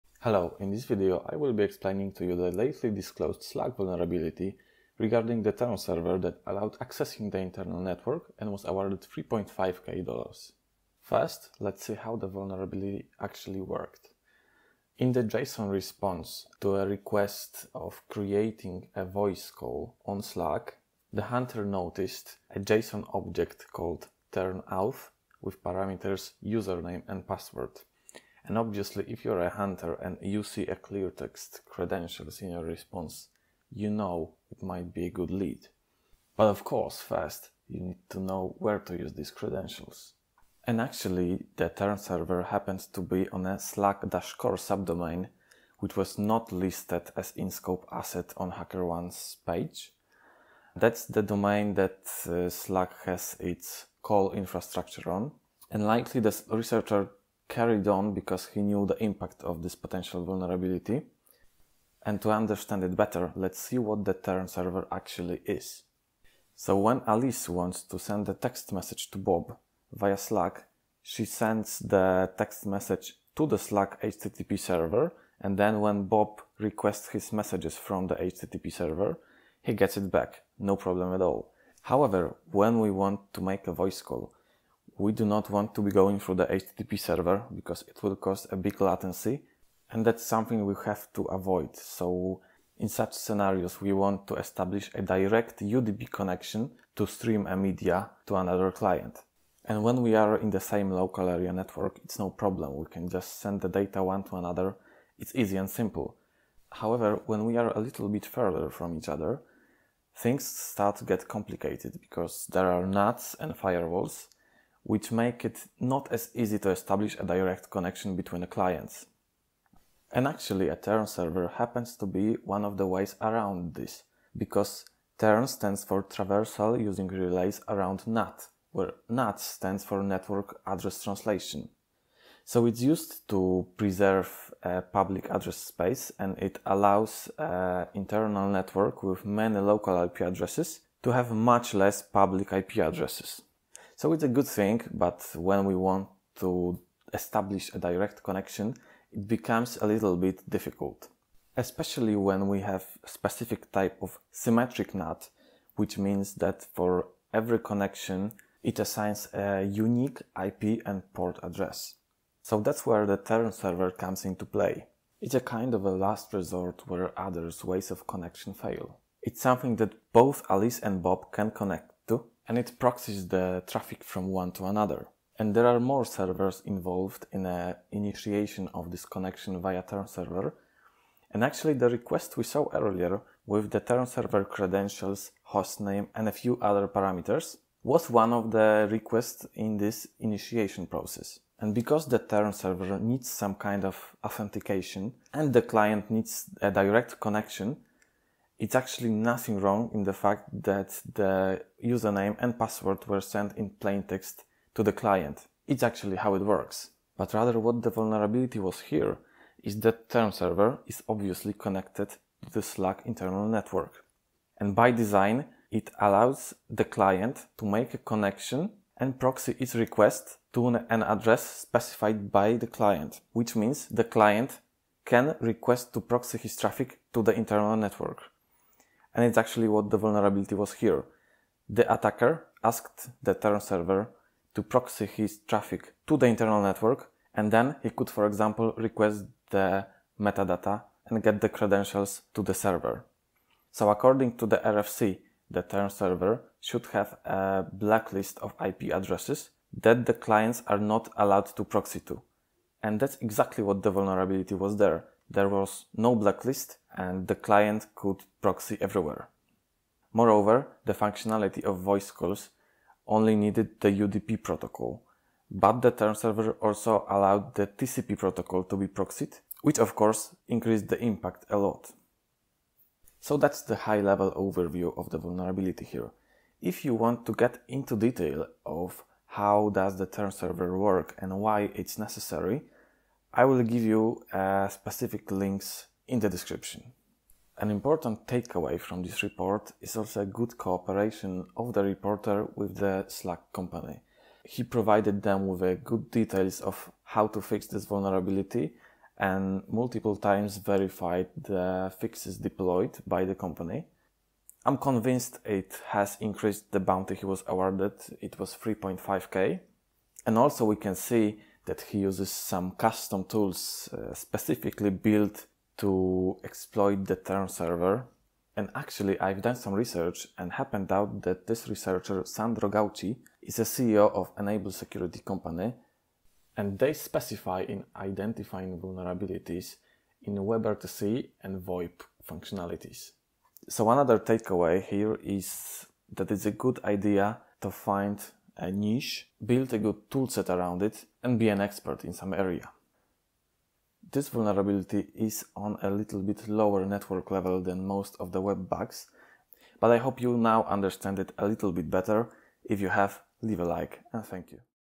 $3,500 Slack SSRF - proxying to internal network via TURN server explained - Hackerone
This video is an explanation of the SSRF vulnerability found by Sandro Gauci and reported to slack on Hackerone.
Links:
* Original report:

00:00 Intro
00:14 password in HTTP response
01:08 what is TURN server?
03:32 What was wrong here?
04:52 Report and reward